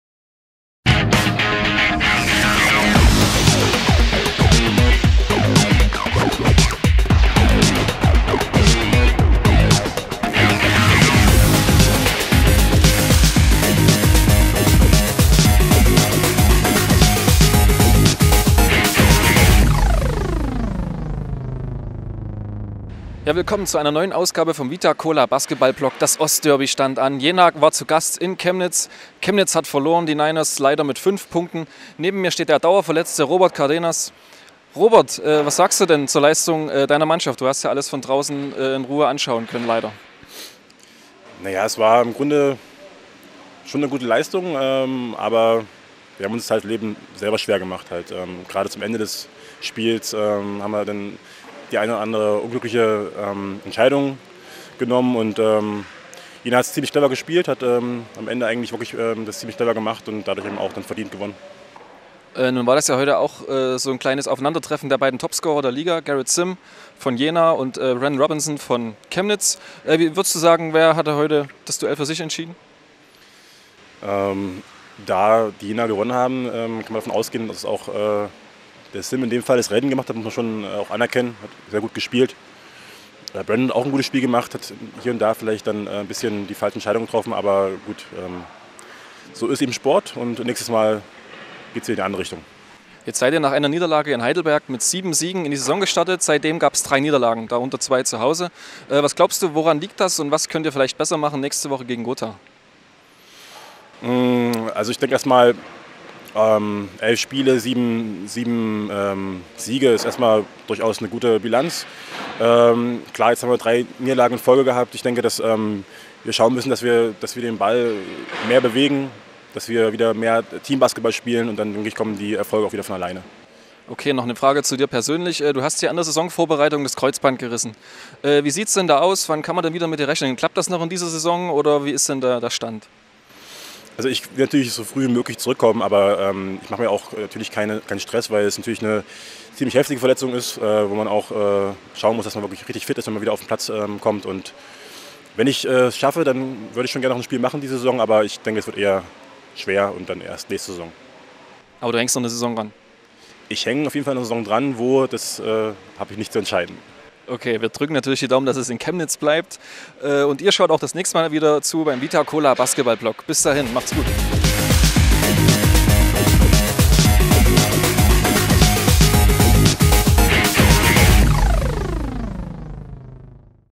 Chemnitz 99 - Saison 13/14 - 5 - Interview mit Robert Cardenas
Robert Cardenas spricht über die Niederlage im Ost-Derby gegen Jena und wie die NINERS wieder auf Erfolgskurs kommen können. Der Power Forward ist wegen eines Kreuzbandrisses noch immer an den Spielfeldrand gefesselt.

Viel Spaß mit dem Interview wünscht dir das VITA COLA Team.